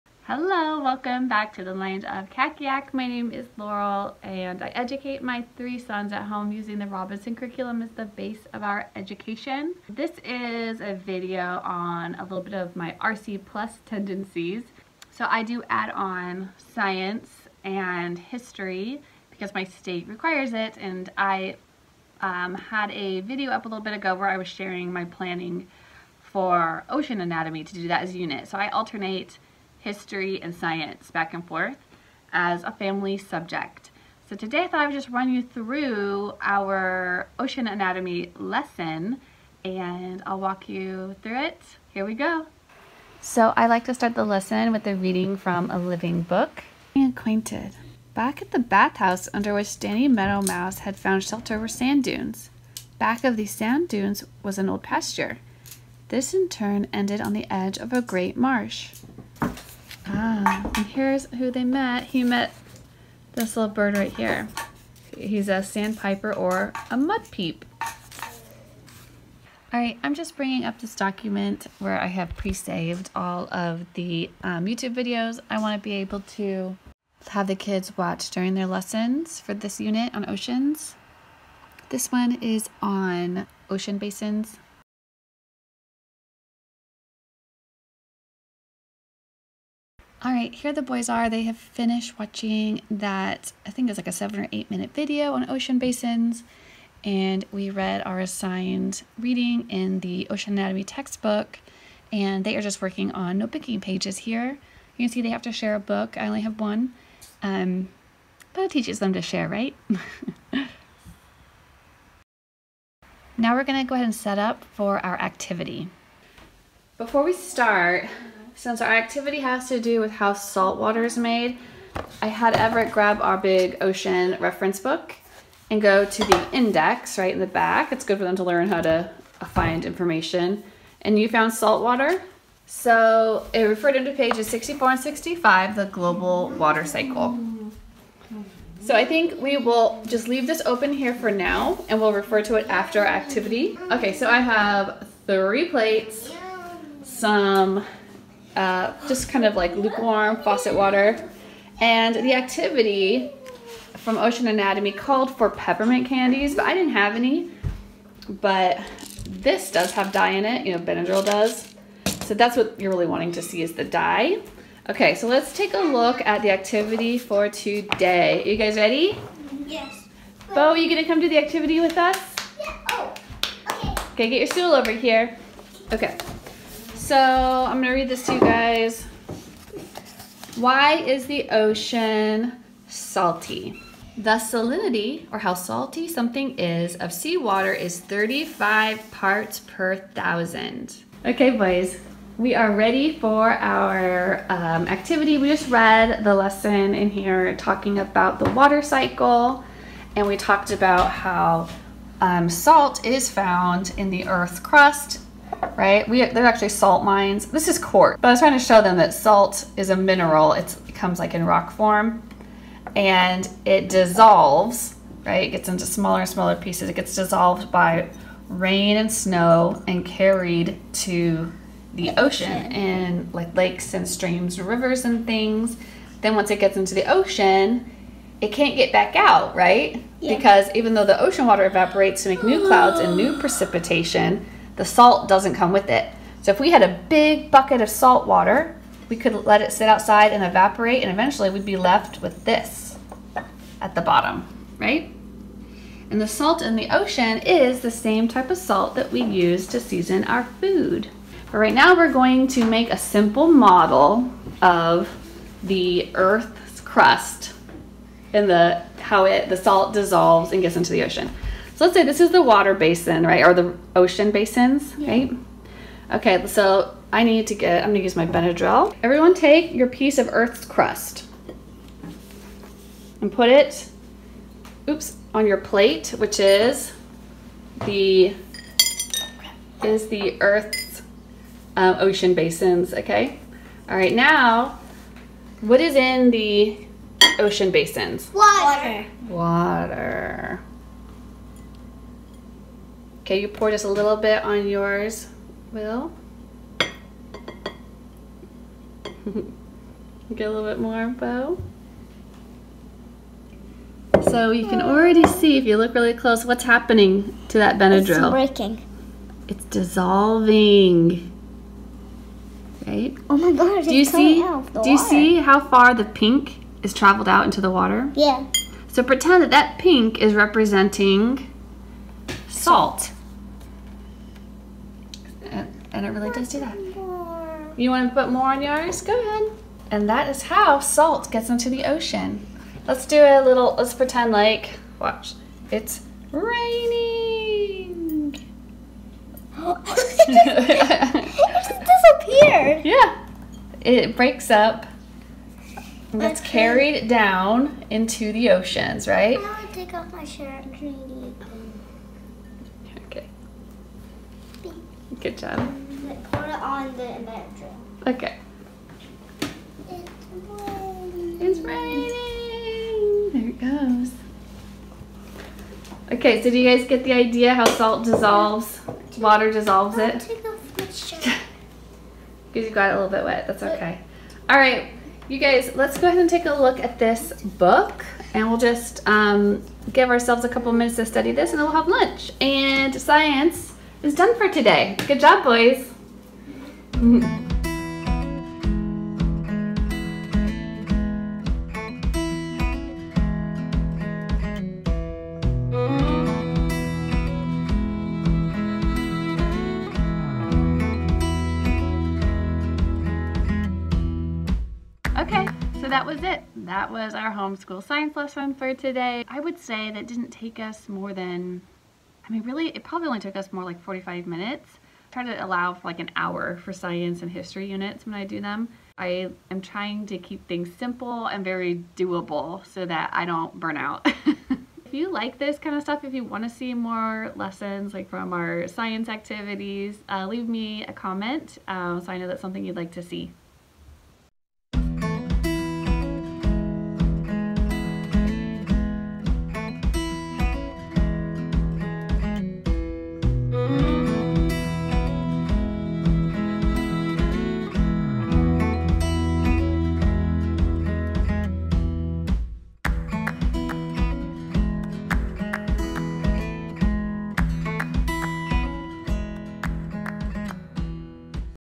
Ocean Anatomy Homeschool Lesson: Family Subject
Today I'm taking you inside a real life lesson. It's sweet and simple, just like homeschool should be.

Find links to today's resources in the document below.

Ocean Anatomy Activity Book